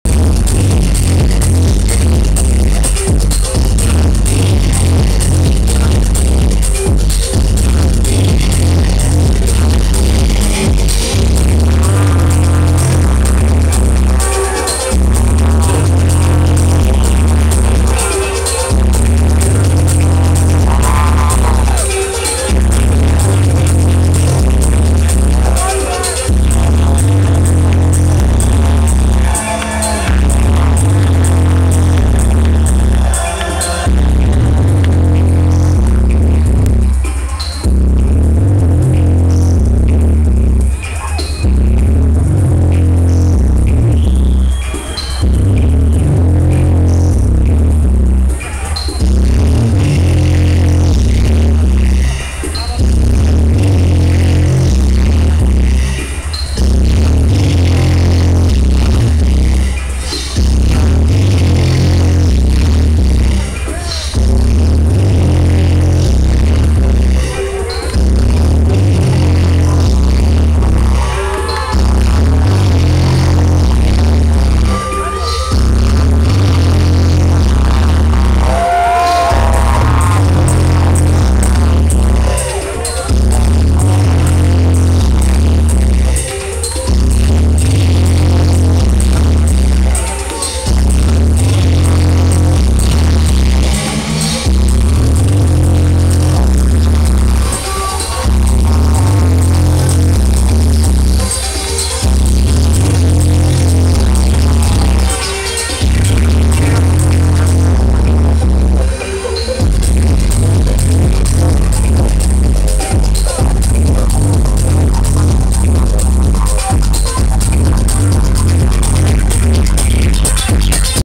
Hector Romero at Club 101 El Paso TX 10-02-2009
Hector Romero at Club 101 El Paso TX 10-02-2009 Satoshi Tomiie getting ready to play